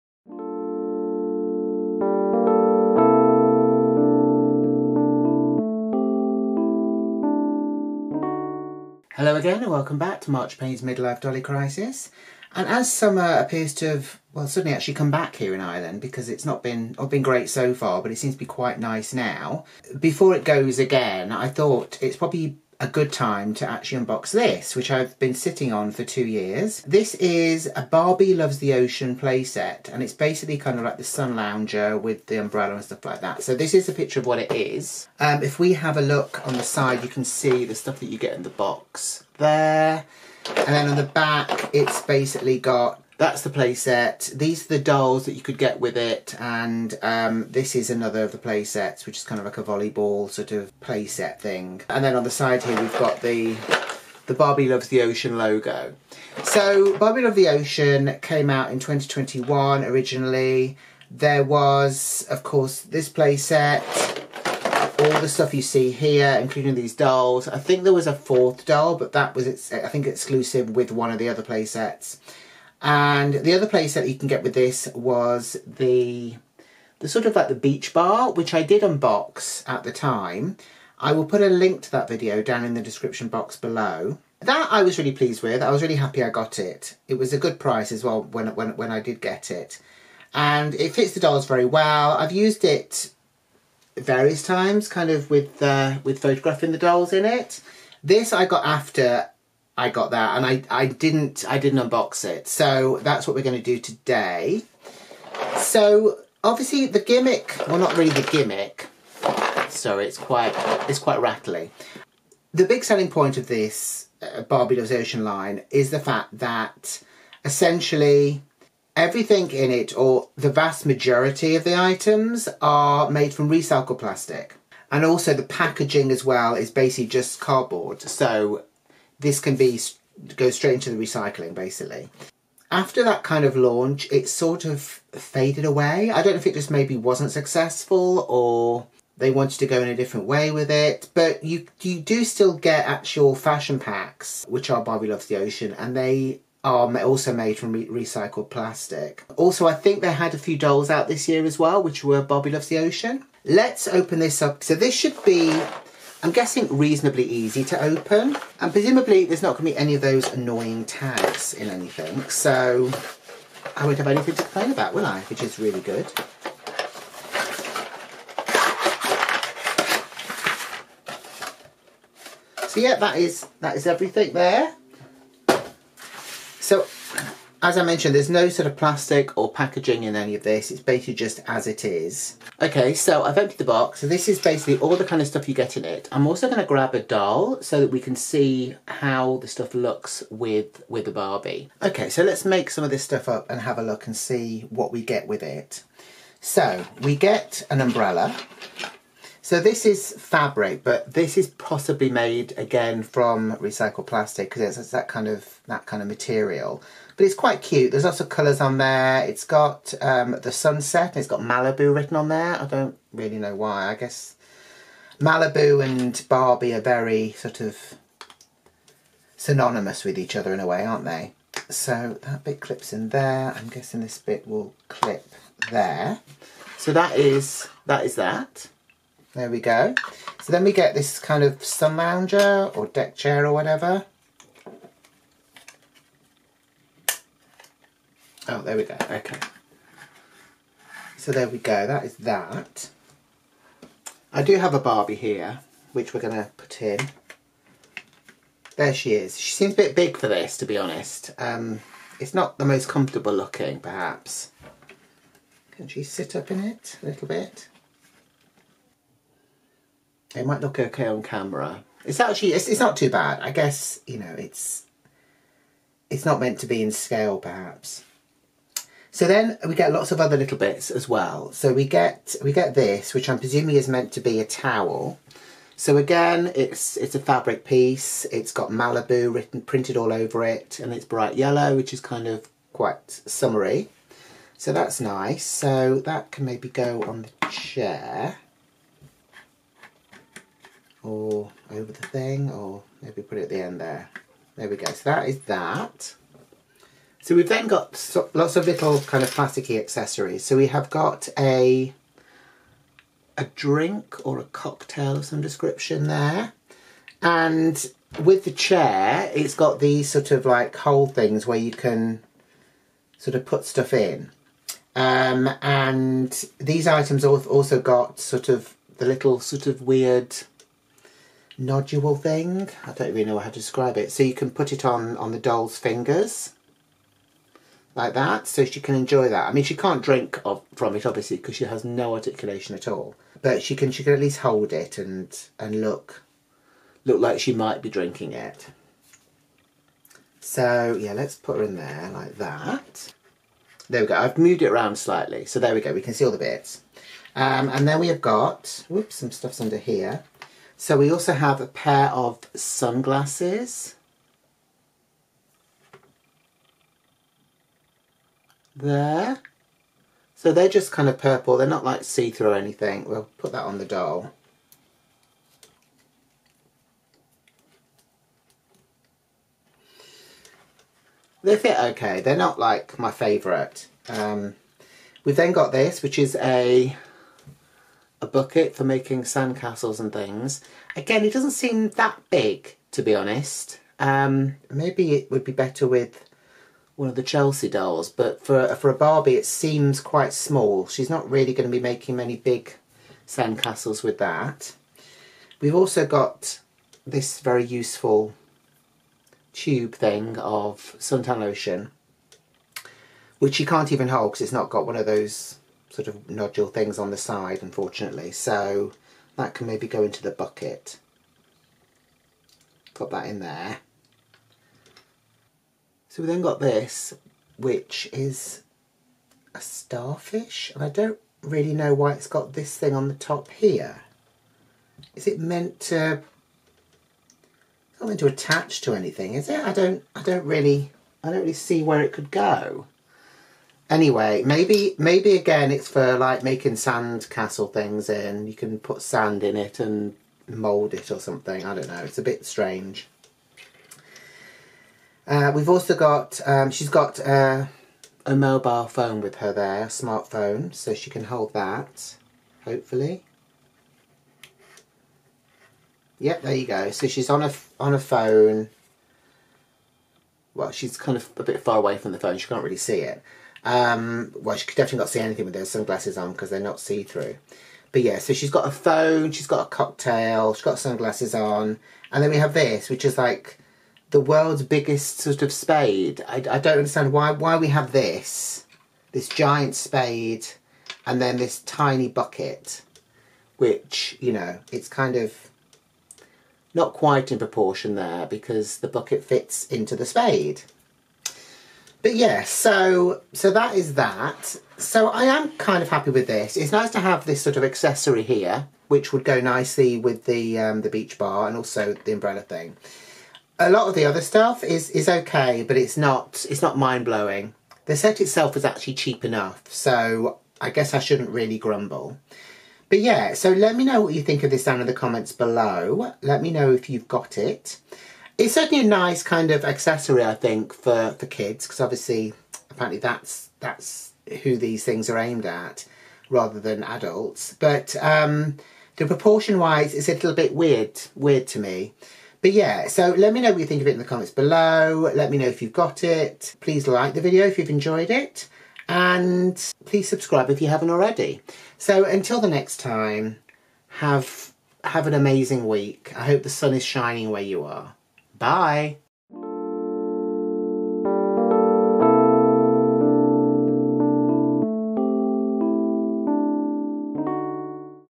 Unboxing Barbie Loves the Ocean Beach Day Playset Mattel *ADULT DOLL COLLECTOR*
This video and channel are aimed at ADULT doll collectors.

There's still time to work on that tan thanks to this Barbie Loves the Ocean "Beach Day" playset!